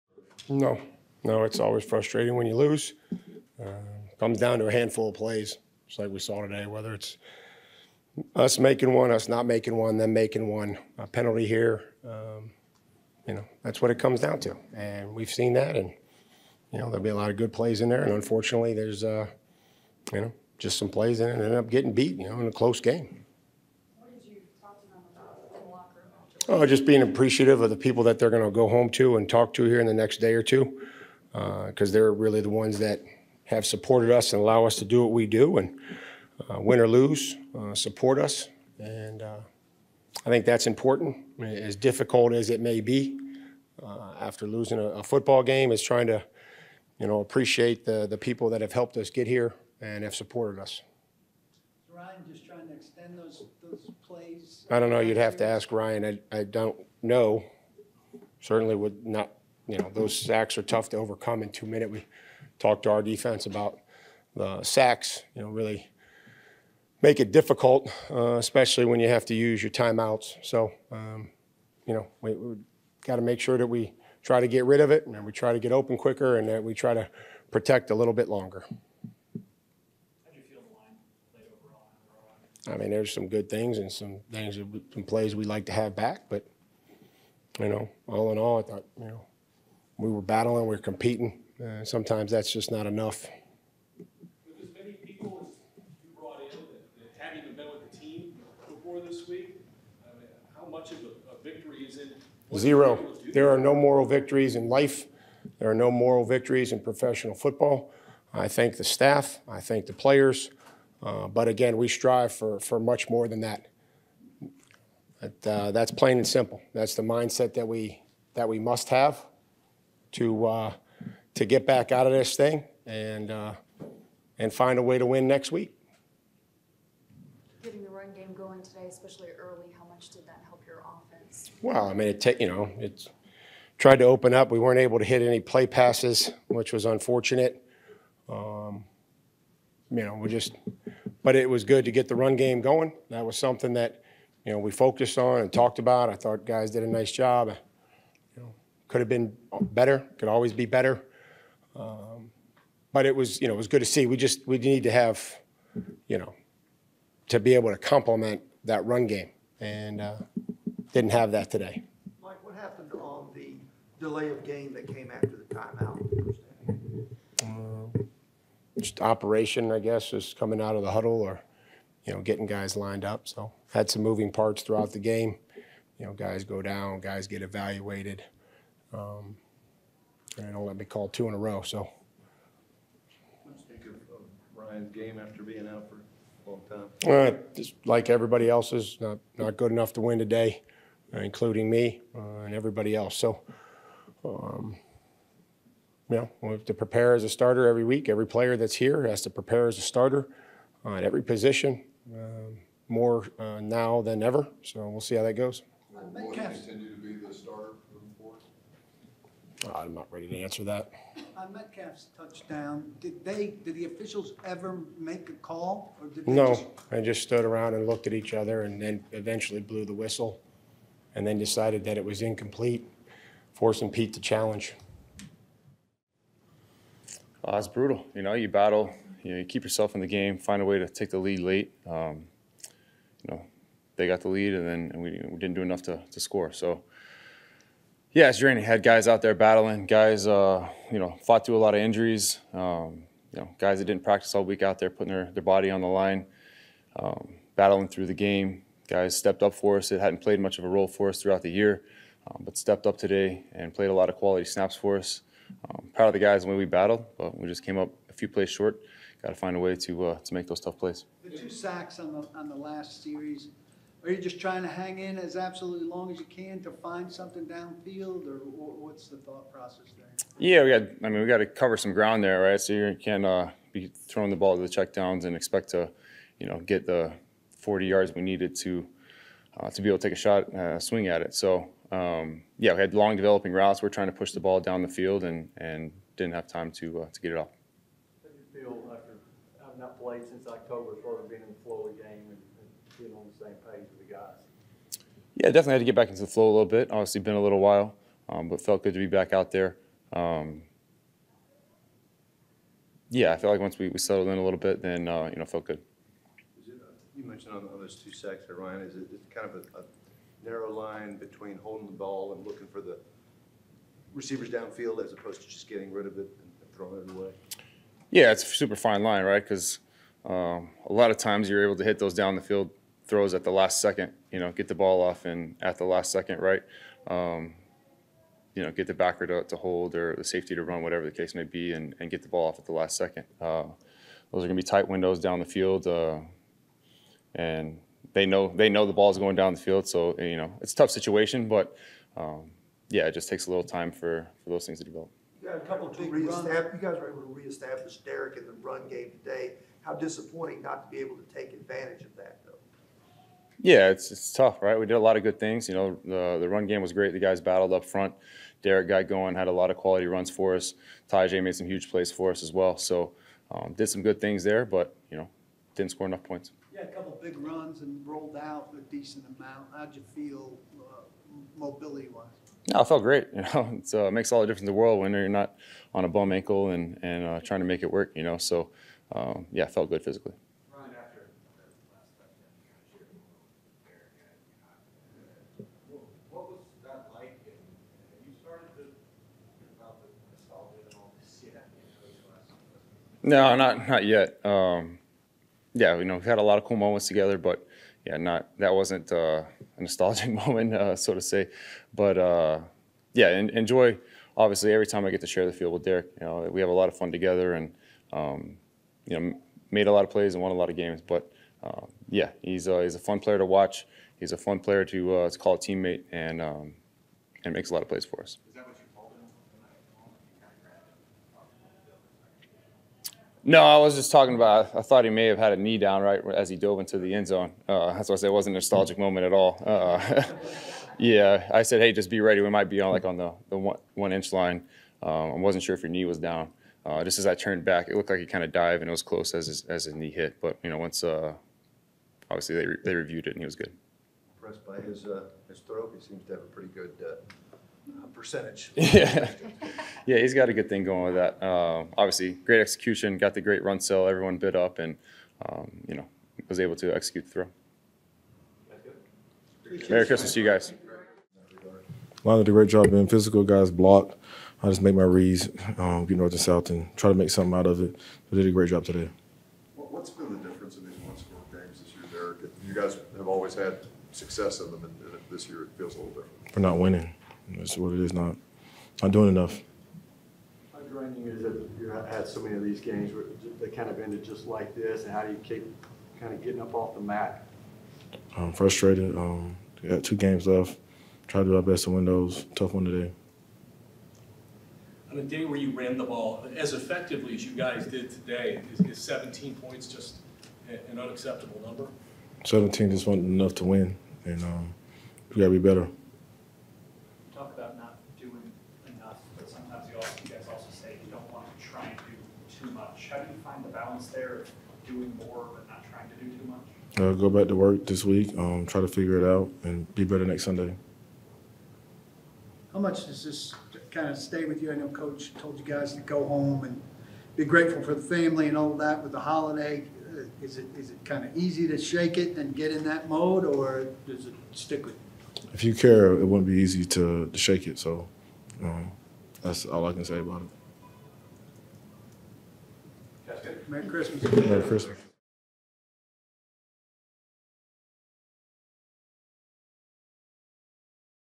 We Aren’t Doing Enough | Media Availability
00:00 - HC Mike Vrabel
03:50 - QB Ryan Tannehill
07:40 - RB Derrick Henry

Titans Head Coach Mike Vrabel, QB Ryan Tannehill and RB Derrick Henry address reporters during a Postgame Press Conference after the Week 16 game against the Seahawks at Nissan Stadium.